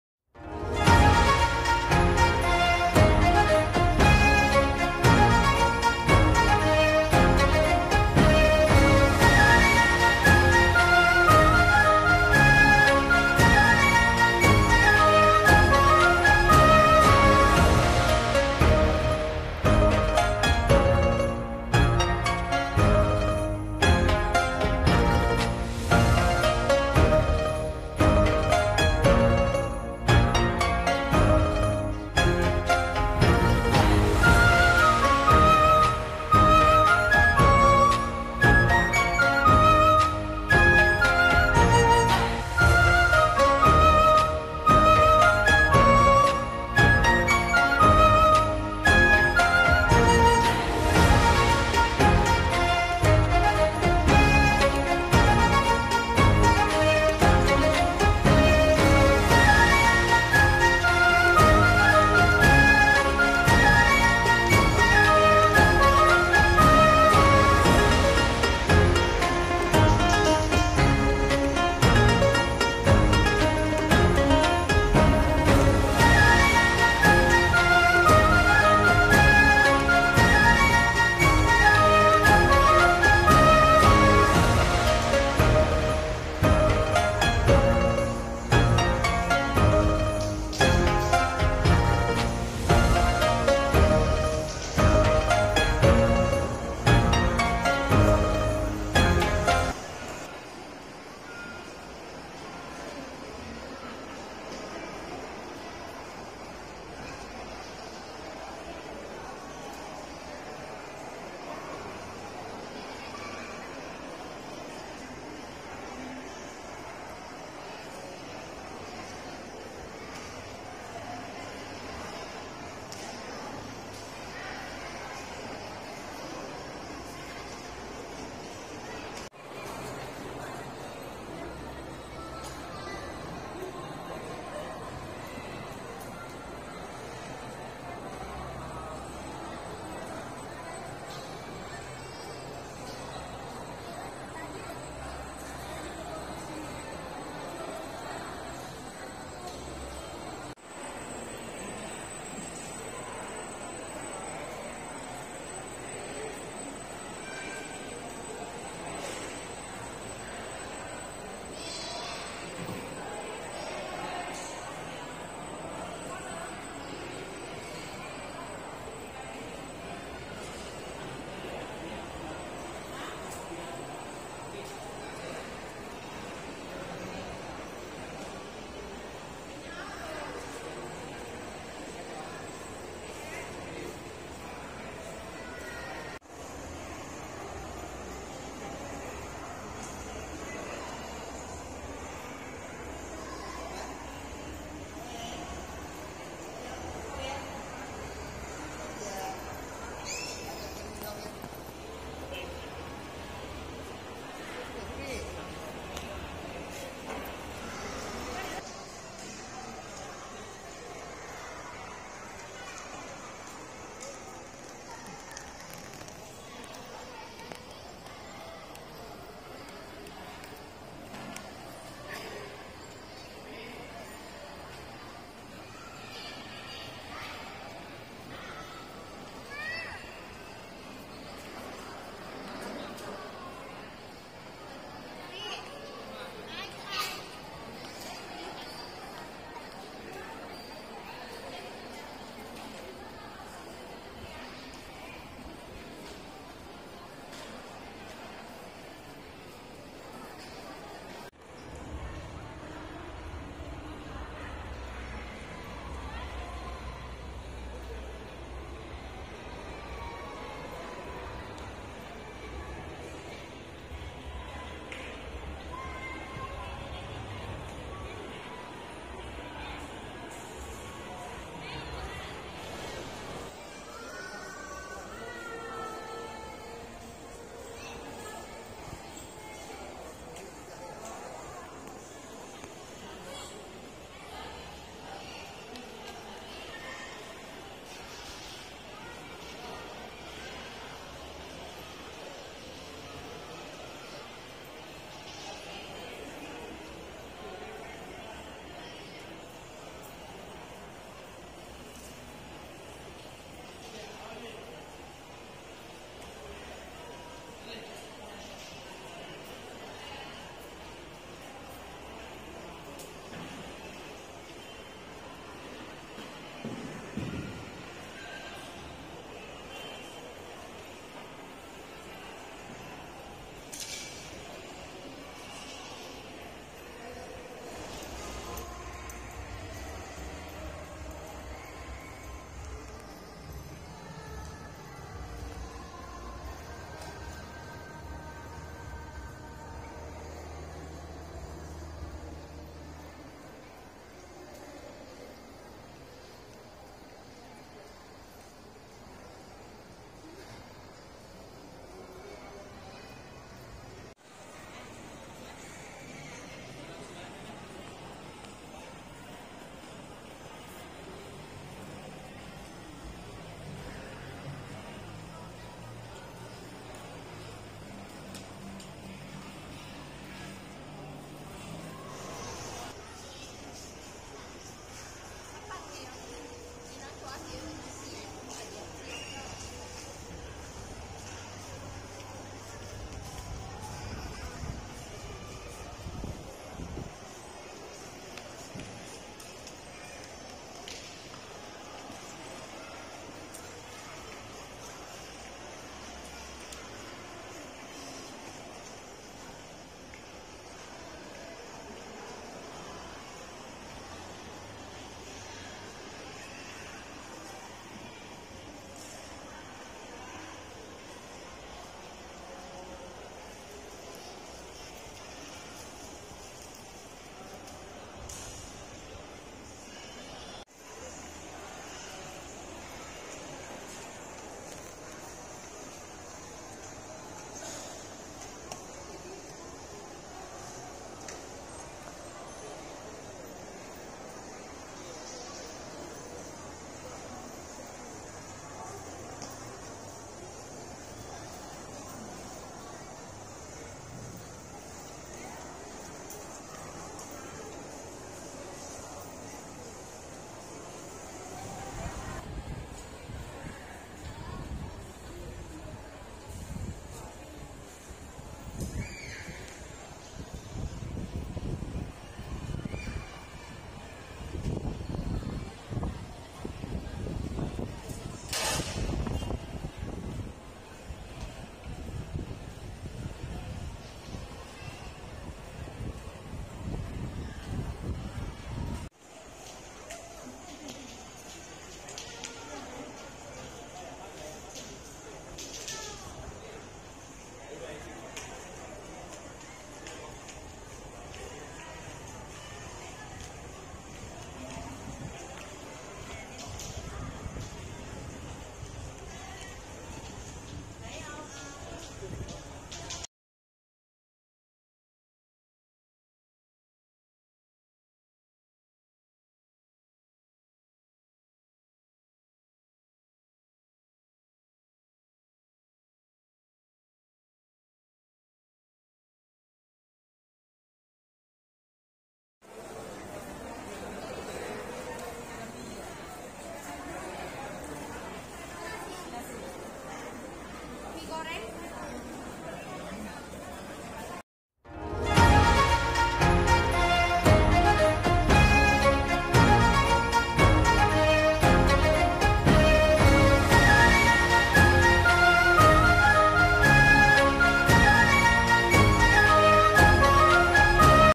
The 2023 Lunar New Year Atmosphere I Maha Vihara Duta Maitreya Batam I @The Year of Rabbit
''Happy Lunar New Year 2023 for everyone who celebrated...
May the new year bring in your life joys that never fade, hopes that never die, and strength that never weakens.
May the Chinese New Year be pleasant and prosperous for you, your family, and your friends.” May the love of God be upon you.

- Location : MAHA VIHARA DUTA MAITREYA, Bukit Beruntung, Sungai Panas, Batam - Riau Islands, Indonesia.

Happy chinese new year 2023 ''GONG XI FA CHAI'' :)